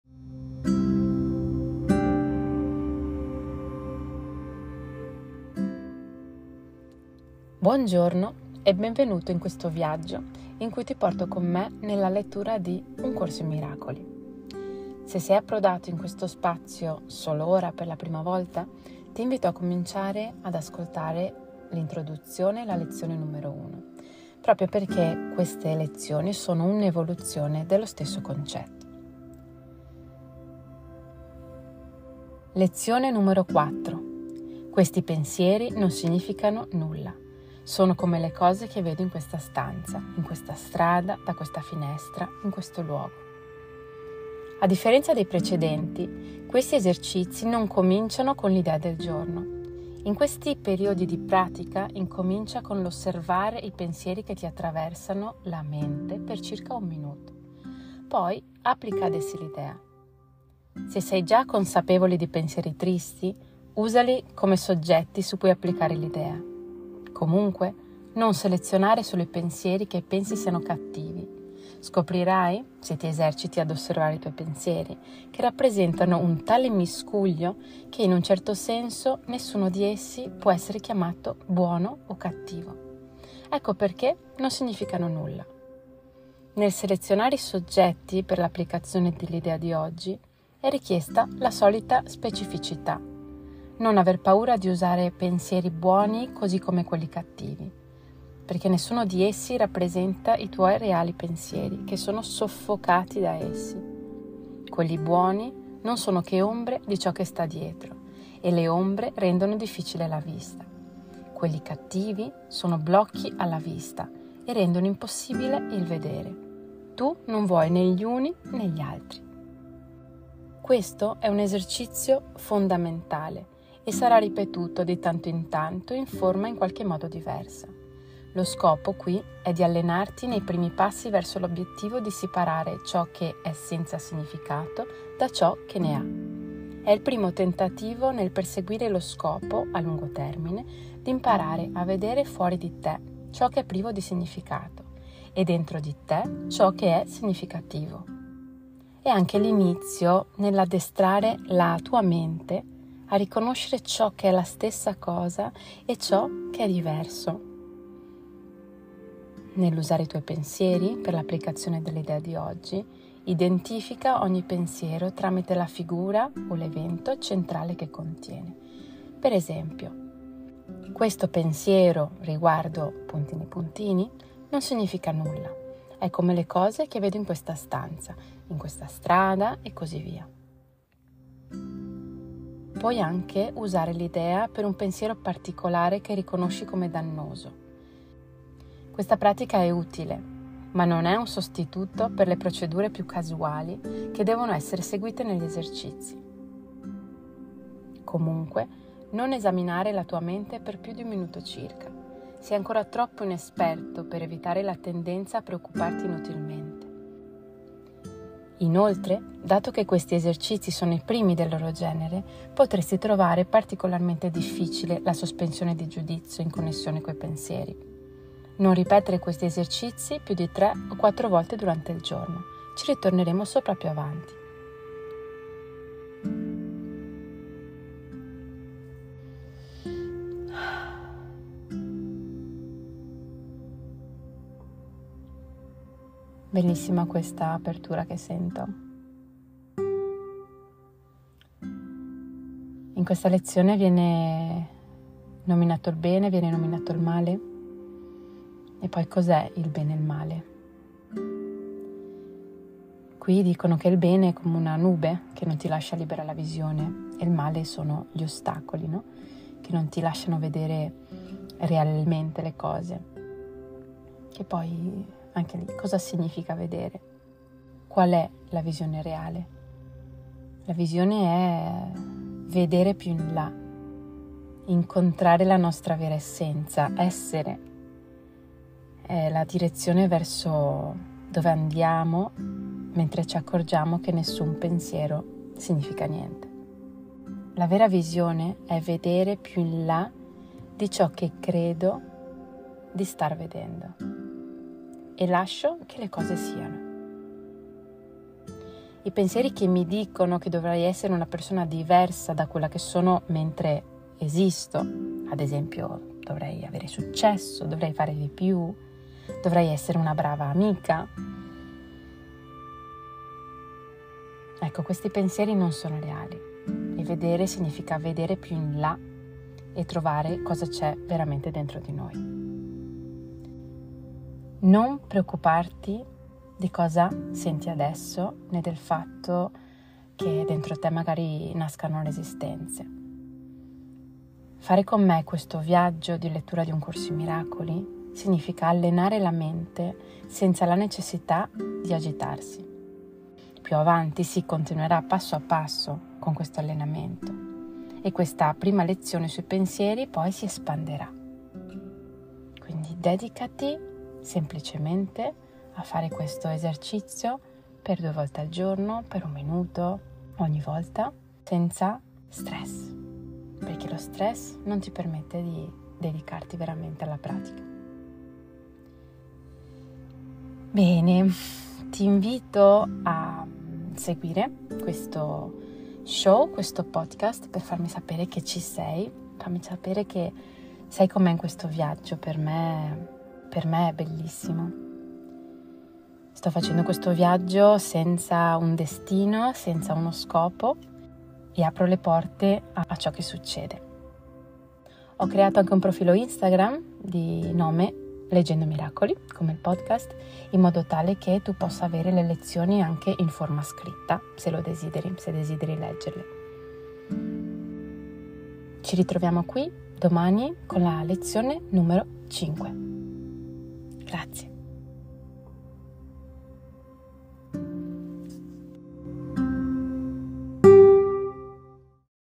Lezione 4. Questi pensieri non significano nulla. Sono come le cose che vedo in questa stanza
Oggi leggiamo insieme la lezione numero quattro del libro “Un corso in miracoli”: QUESTI PENSIERI NON SIGNIFICANO NULLA. SONO COME LE COSE CHE VEDO IN QUESTA STANZA.

Puoi trovare tutte le lezioni scritte nel profilo Instagram che ho appena creato @Leggendomiracoli